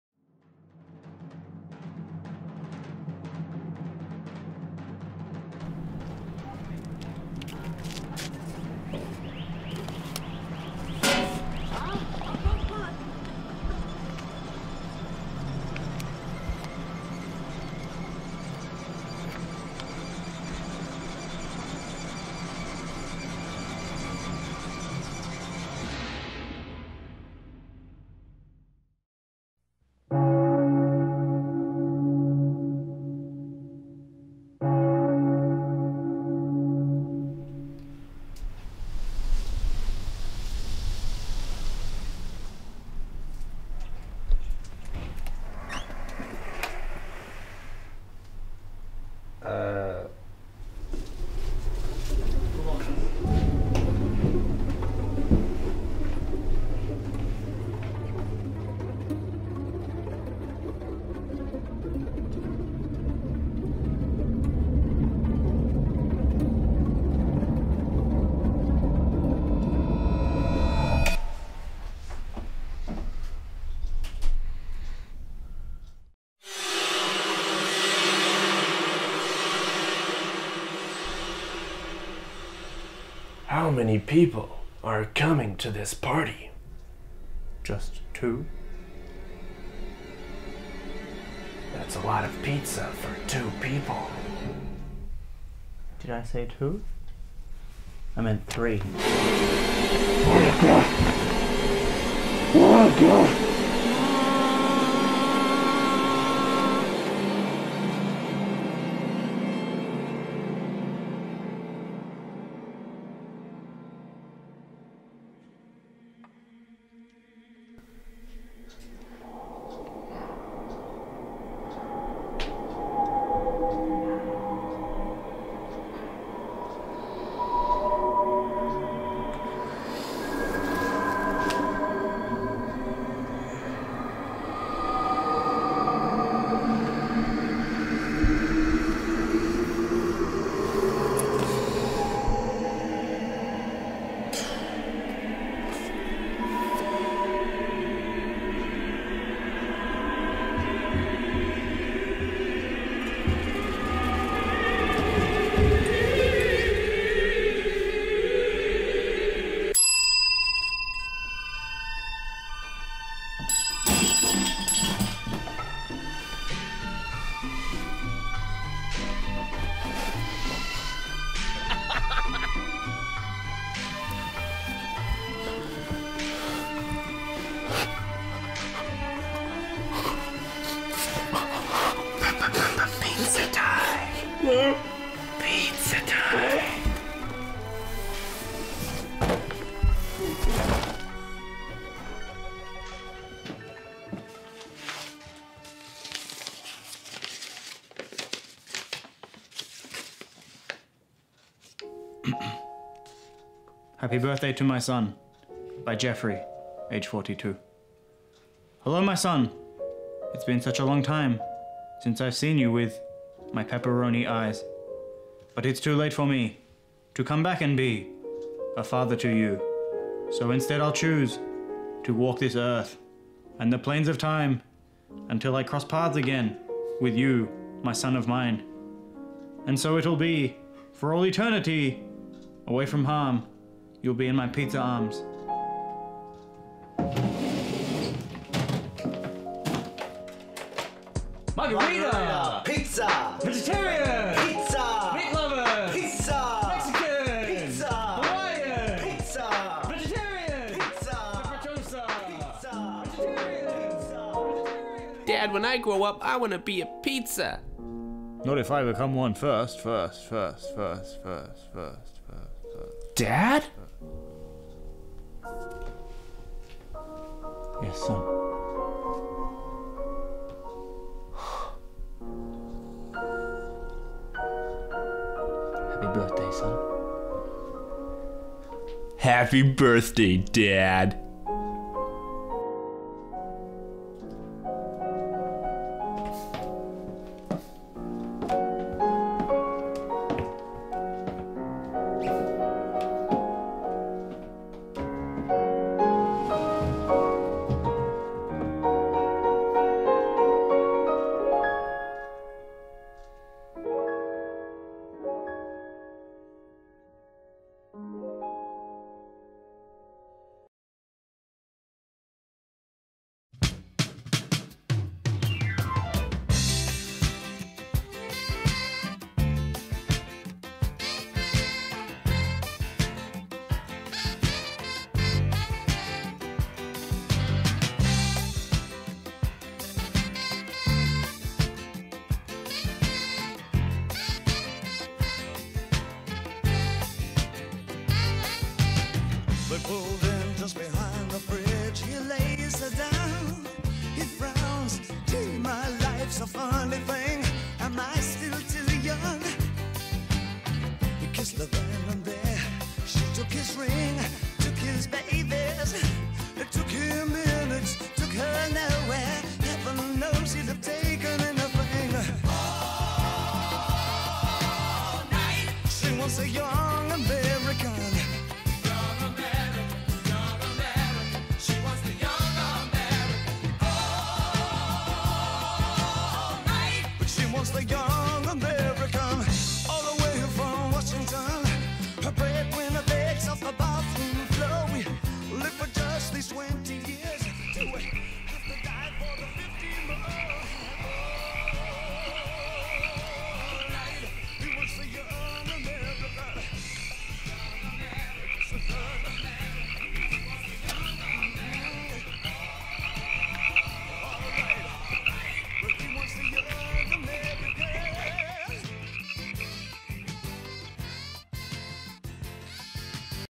Pizza Party
Trent Smith gets more than he bargained for when he decides to attend... the Pizza Party.